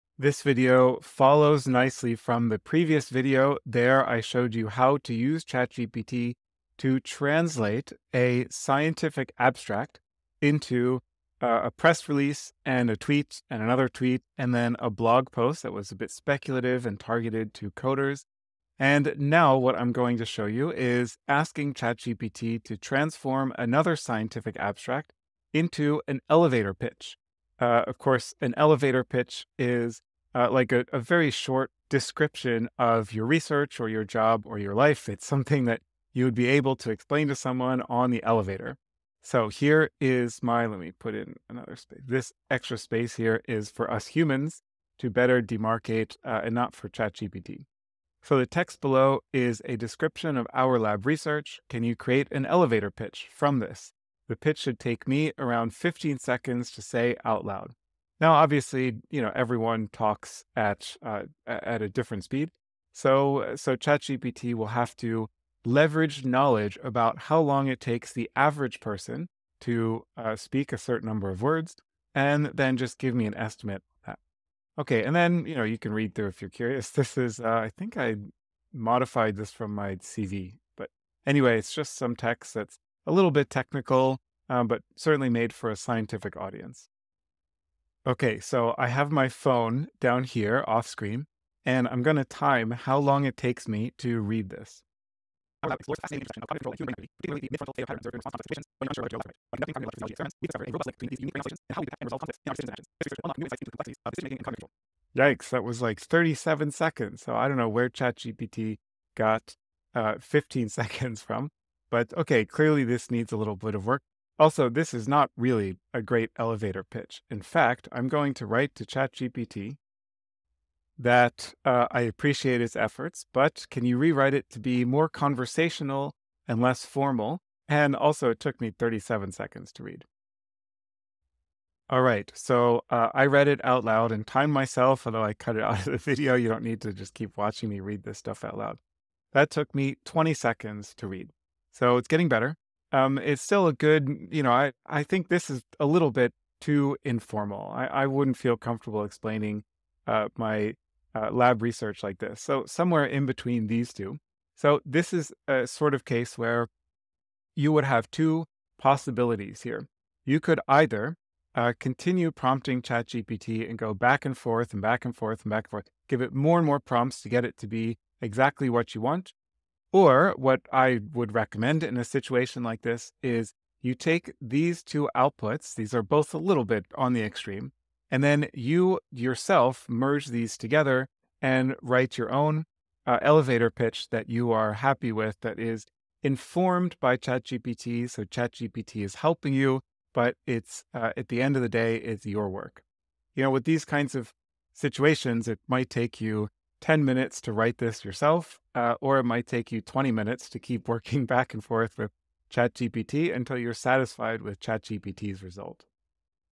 ChatGPT can help you transform academic/scientific abstracts into press releases, tweets, blog posts
This is a preview video from a 4-hour course on using ChatGPT to help you write, focusing on academic and scientific contexts.